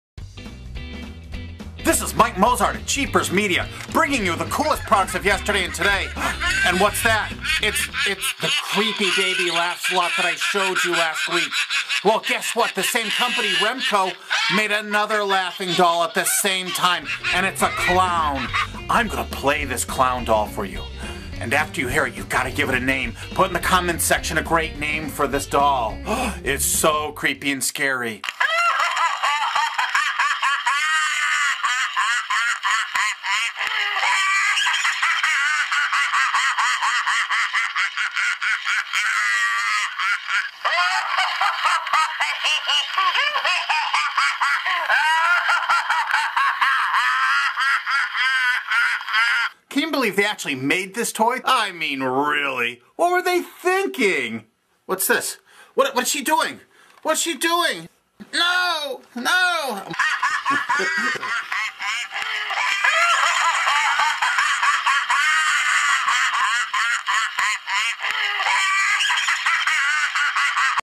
Scary Clown Doll Funny LoL Videos Funny Video Fail Toy Review Mike Mozart of JeepersMedia on YouTube
Halloween 2011 Freddy Kruger Nightmare on Elm Street Fail Toy Doll Review by Mike Mozart of JeepersMedia on YouTube. This is the Famous Doll of Freddie Kruger that Has the PeeWee Herman Doll voice Mechanism due to a Factory Mistake!